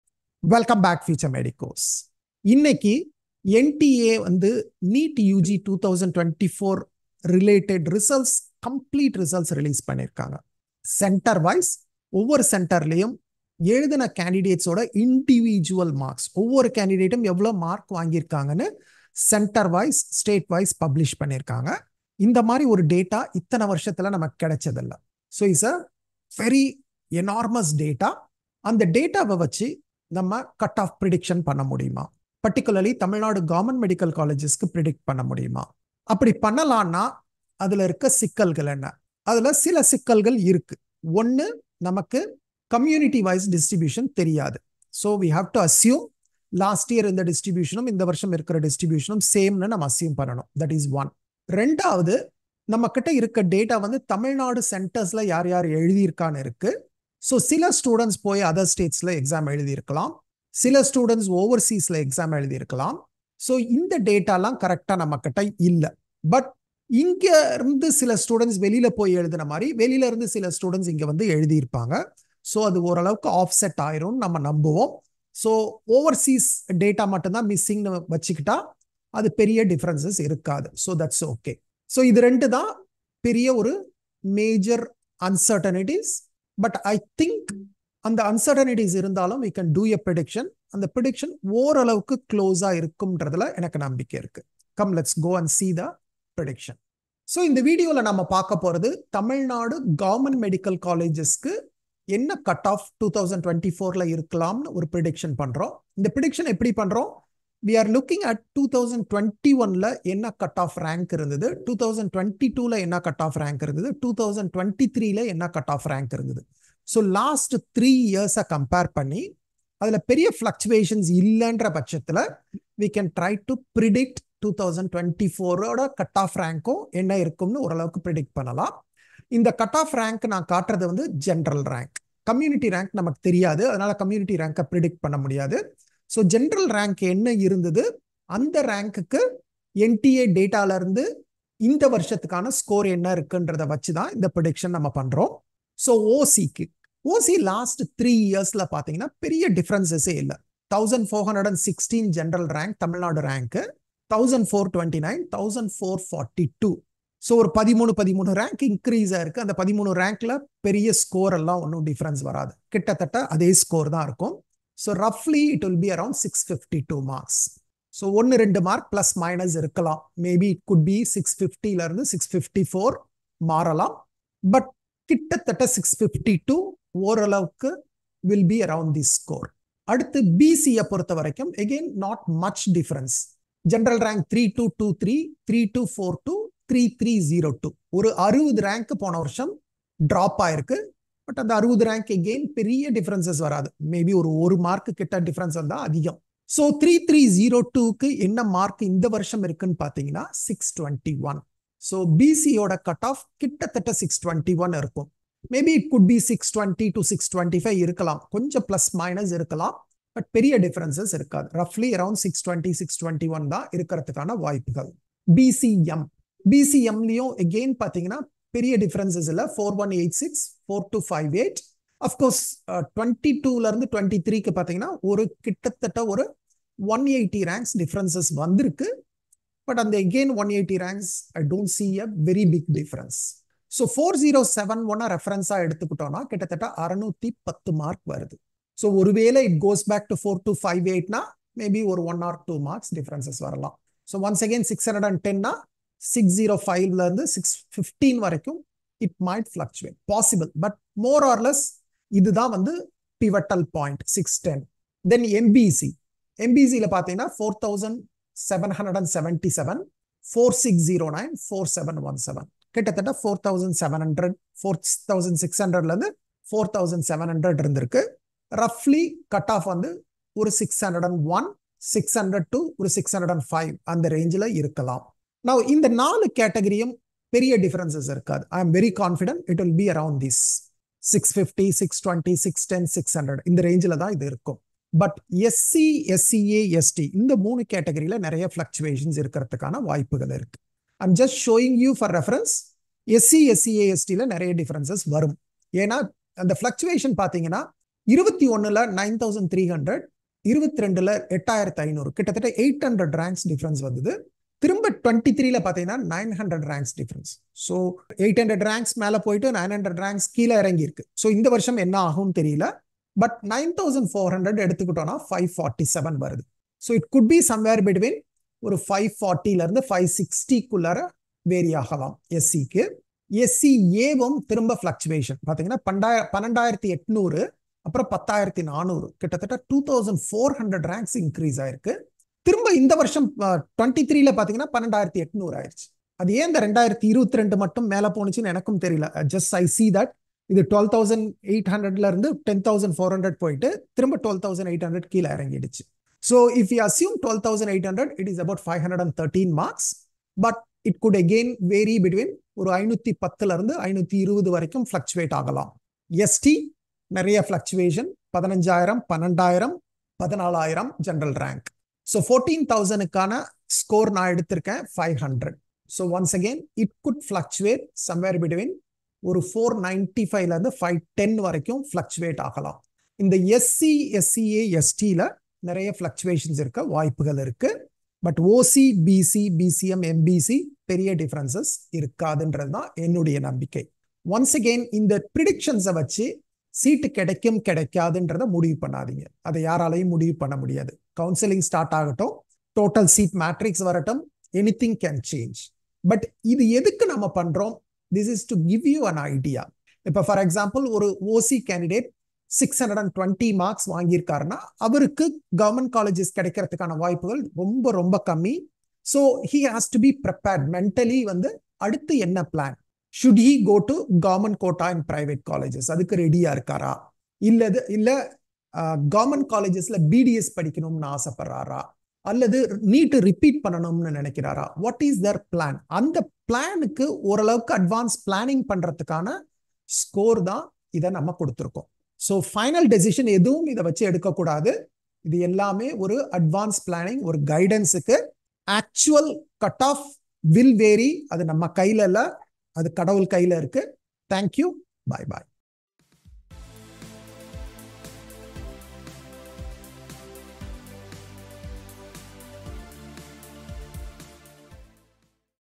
Predicted Cutoff for Tamil Nadu Govt Medical Colleges 2024
NTA has released a very detailed data for NEET UG 2024. Prior to this data release, every candidate in Tamil Nadu knew their NEET score and NEET all India rank. However, they were not able to predict their position in Tamil Nadu or what’s the possibility of getting an MBBS seat at a government medical college in Tamil Nadu. This is where this video might help you.

Every year, Times of India publishes some useful data related to the NEET ranks in Tamil Nadu. They publish how many candidates are there in each band of marks (typically the band is either 50 marks or 100 marks). But, this year, we need not look for TOI data as NTA has directly given this data.

I have analysed the data and predicted the cutoff marks for MBBS seats at government medical colleges in Tamil Nadu. I have explained the assumptions and the process through which I predicted the cutoff. Please watch the video. You may find it useful for planning.